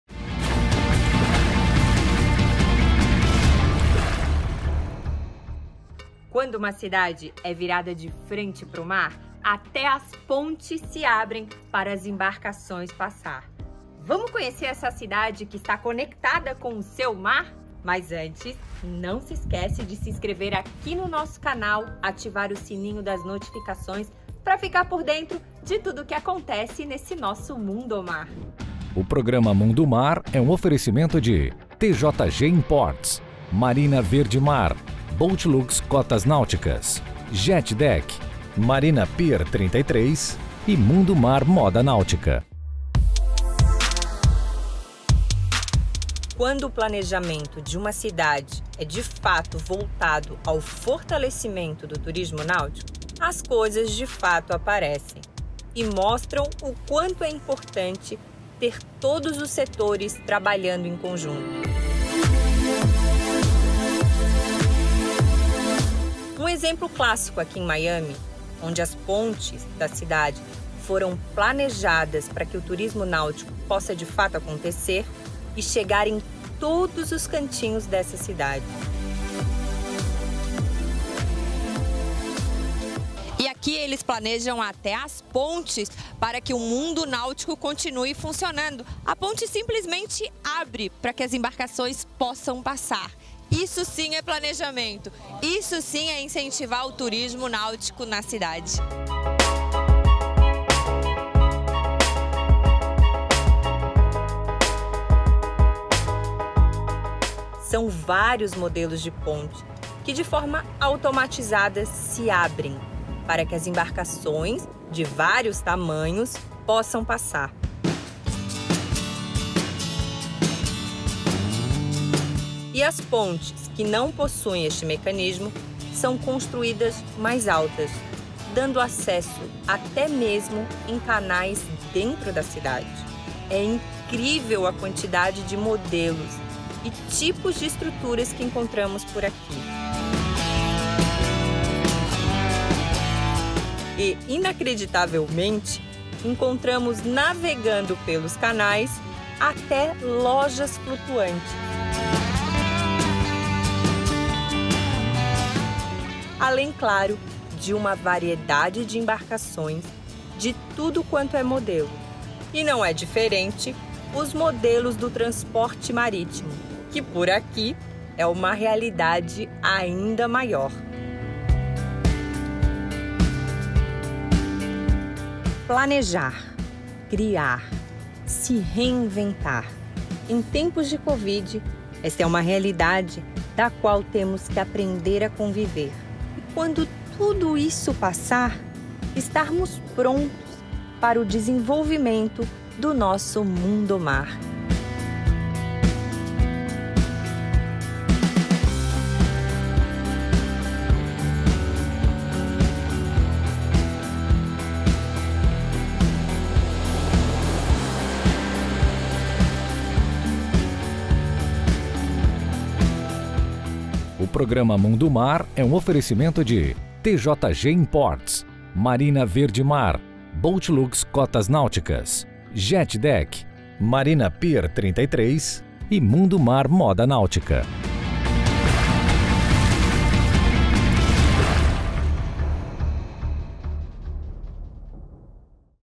As Pontes Náuticas de Miami
Quando o planejamento de uma cidade é de fato voltado ao fortalecimento do turismo náutico, as coisas aparecem e mostram o quanto é importante ter todos os setores trabalhando em conjunto.
Da uma olhada por este angulo a cidade de Miami